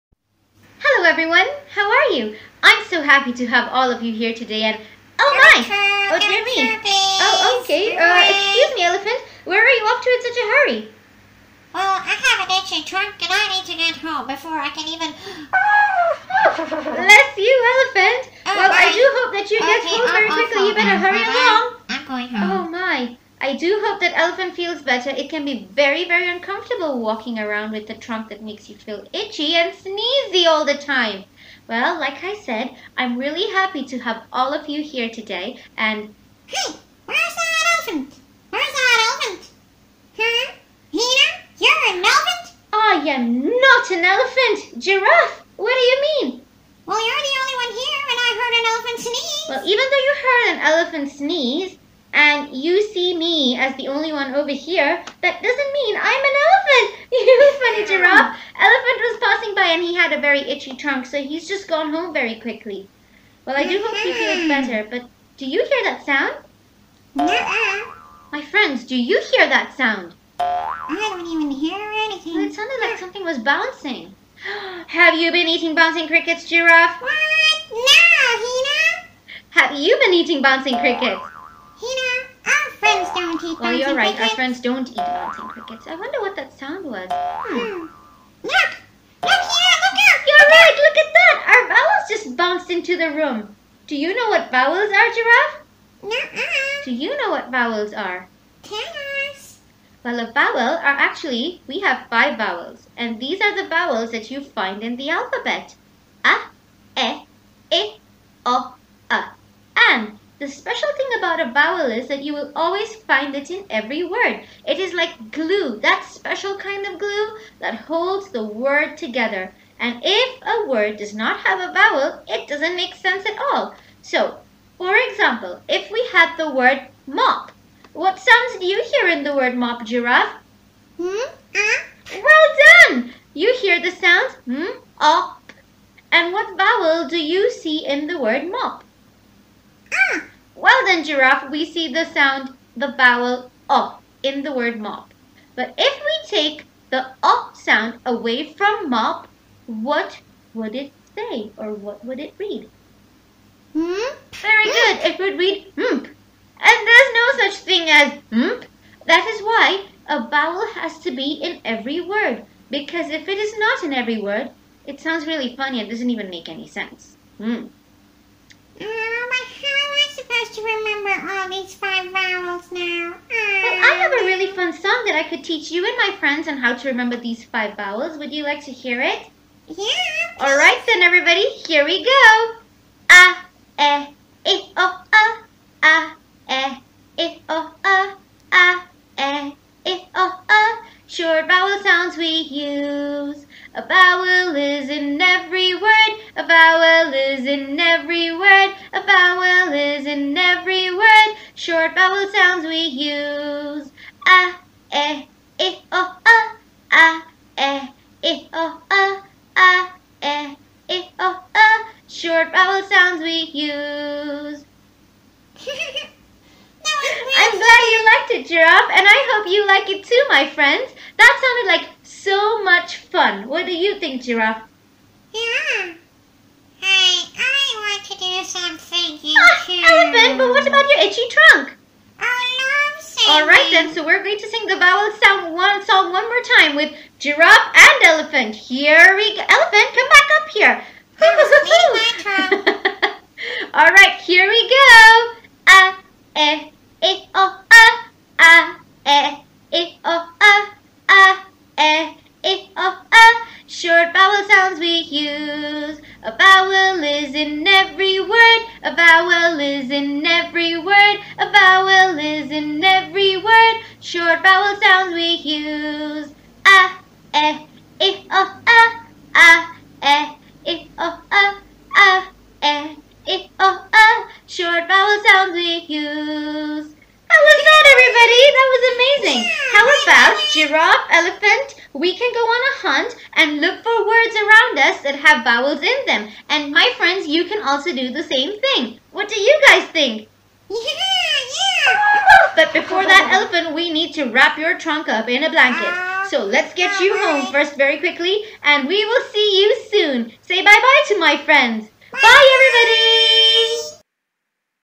Vowel Sounds - Diamonds Week 3 and 4
This video explains what vowels are and why they are in every word. Join in and learn the fun vowels song towards the end of the video to remember the vowels!